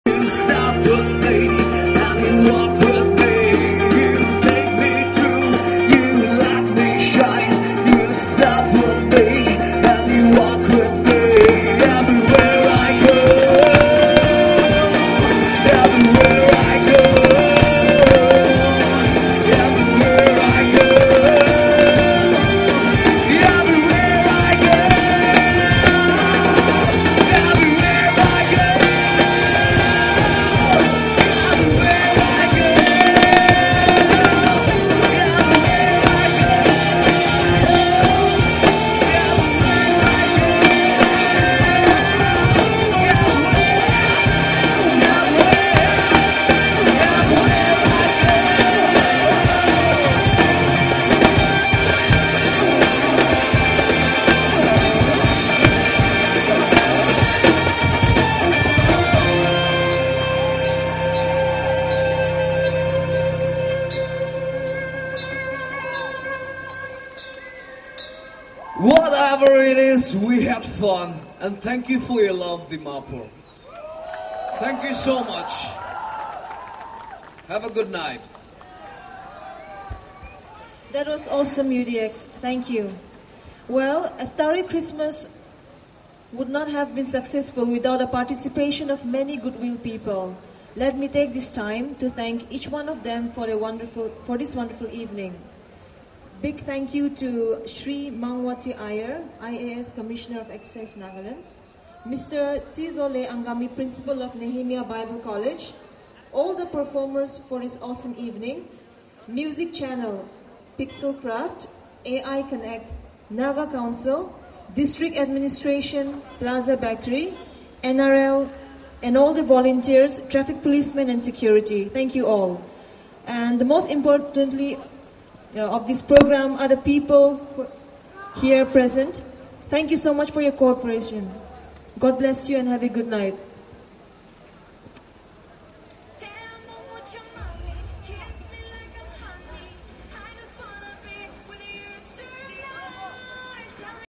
UDX Nagaland in concert
UDX at "A Starry Christmas" open air concert @ City Clock Tower, Dimapur